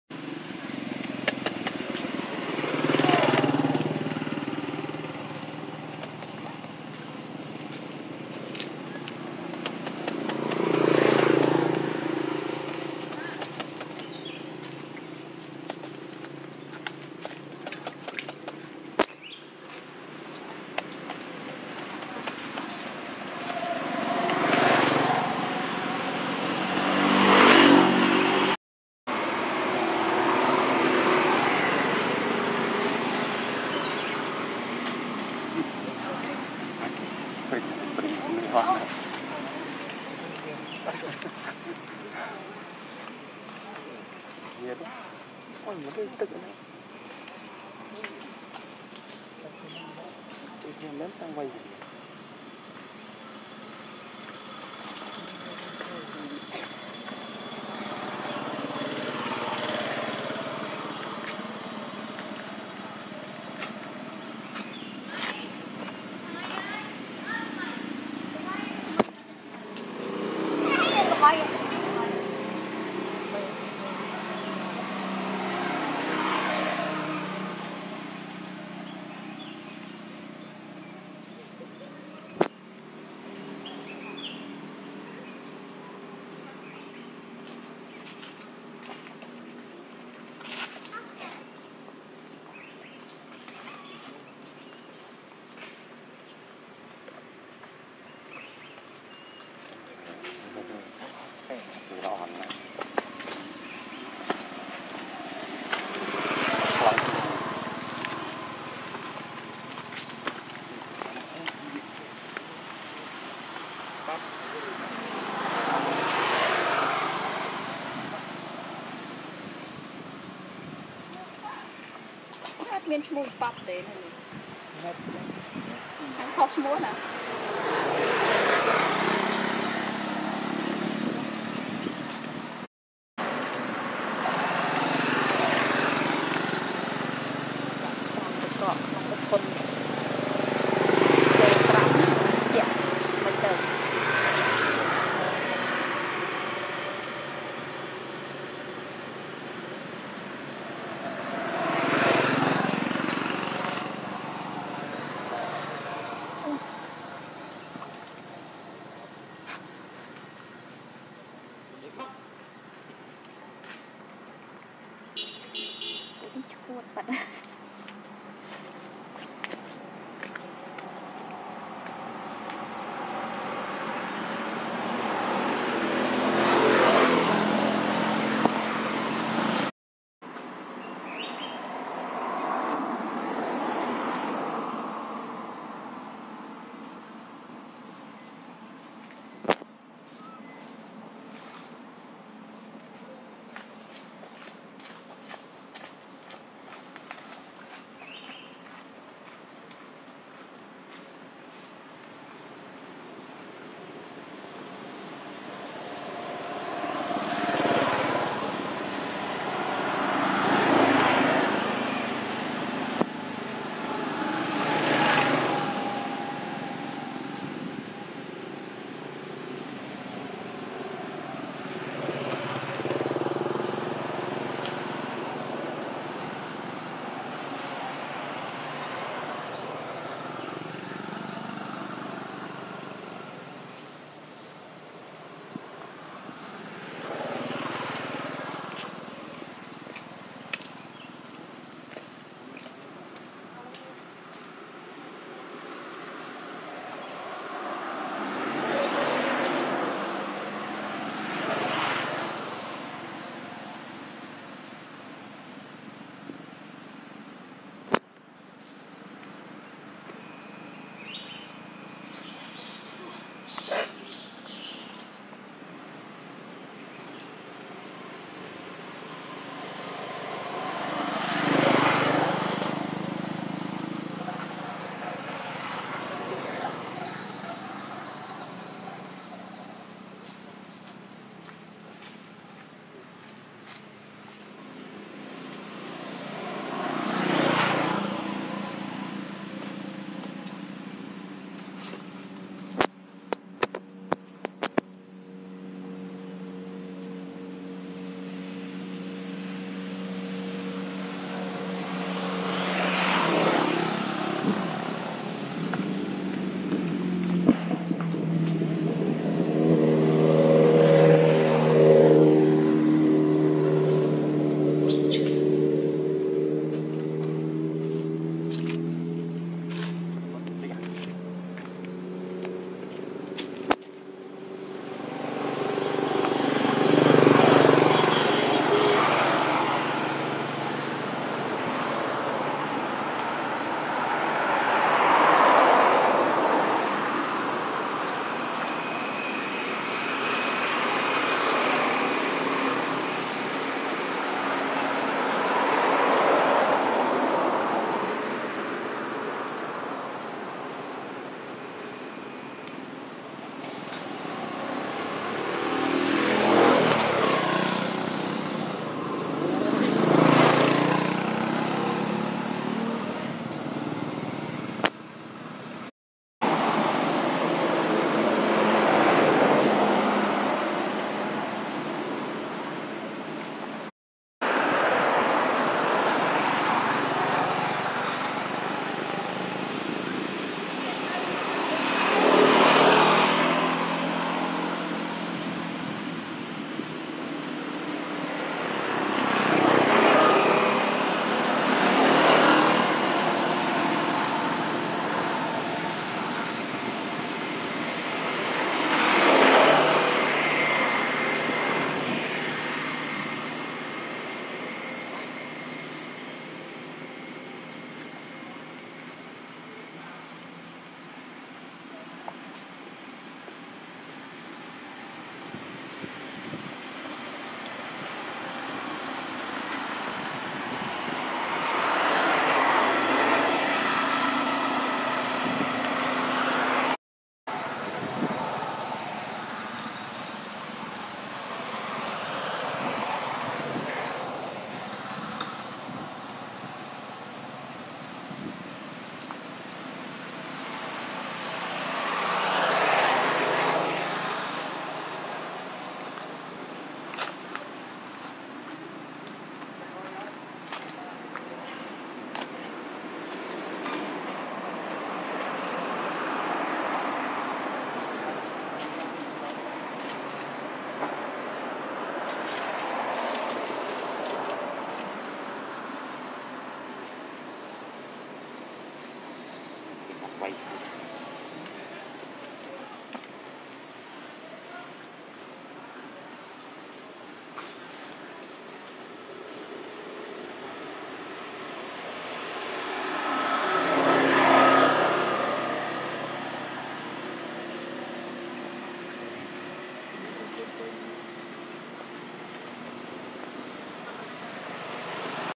Baby Monkey Bobby Want To Free But Mom Brinn Need To Strict With Him
Thanks